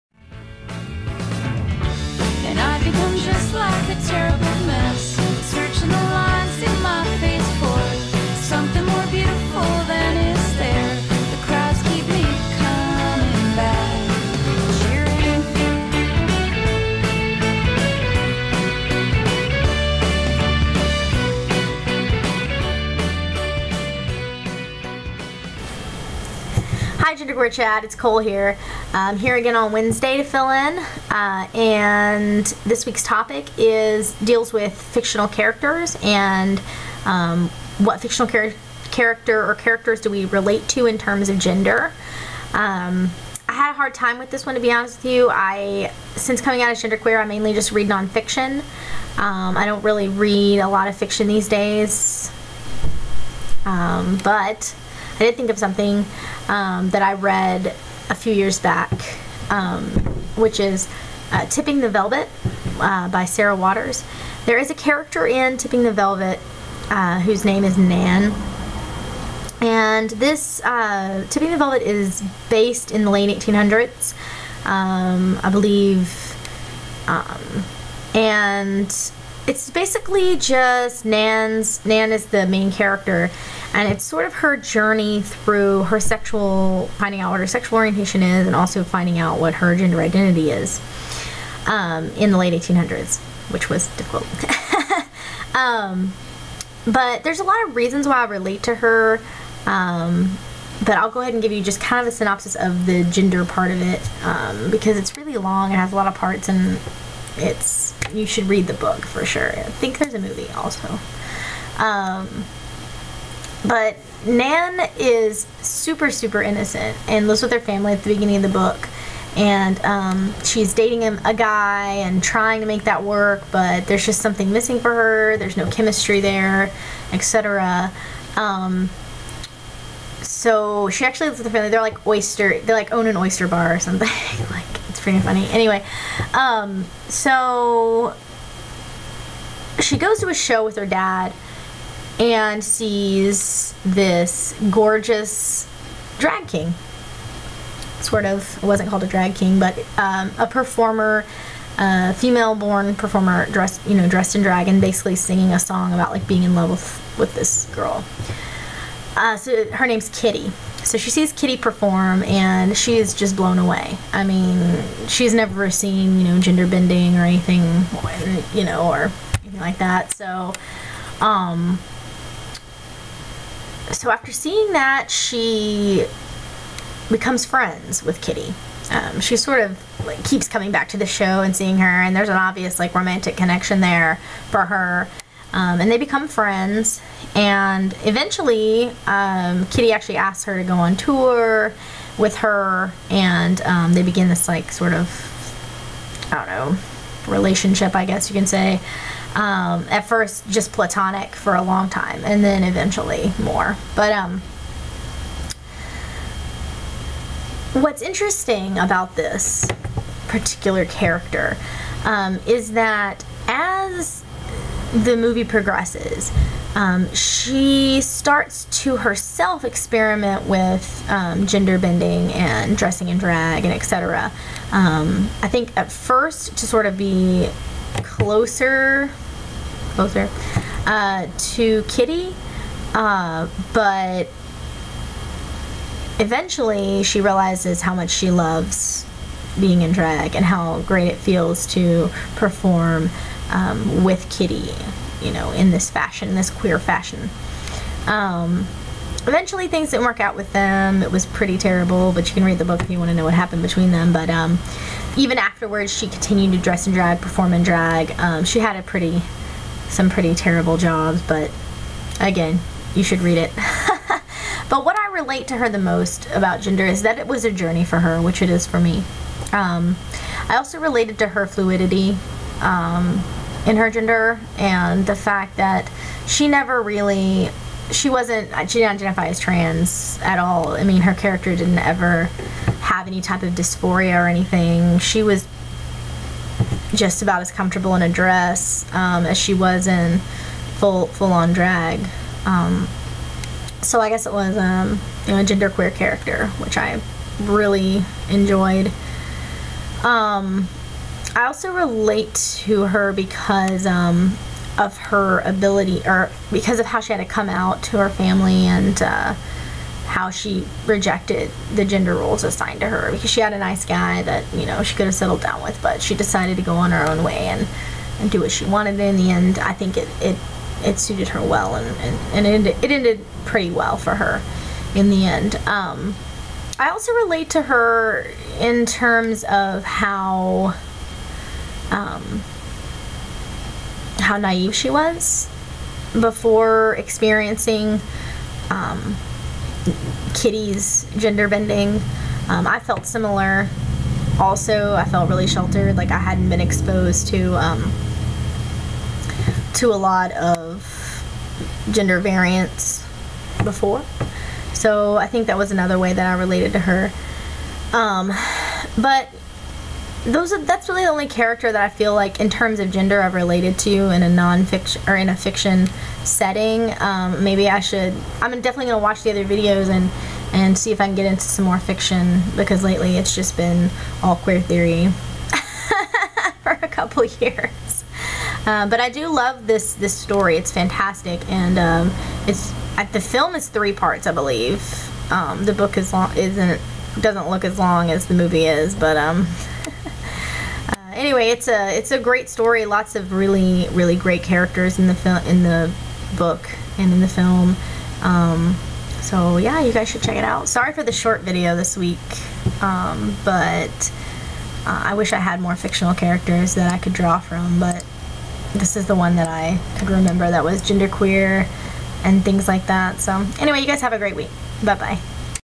Fictional Characters related to gender
I am describing a fictional character, Nan, from Tipping the Velvet and how I relate to her.